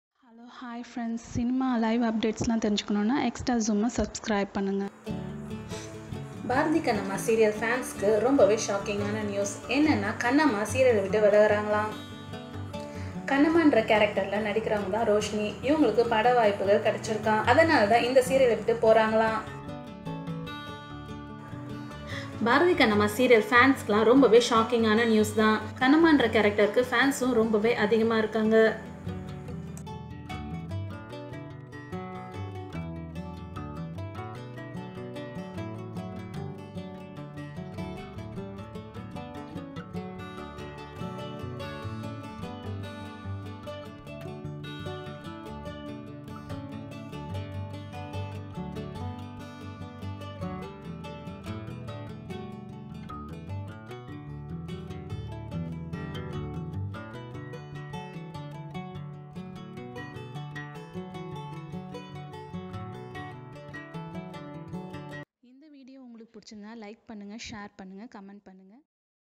Shocking: Bharathi Kannamma Serial Roshini Quit From Bharathi Kannamma Serial | Reason | Latest
Shocking: Bharathi Kannamma Serial Roshini Quit From Bharathi Kannamma Serial |  Reason

Bharathi kannamma சீரியலை விட்டு விலகும் கண்ணம்மா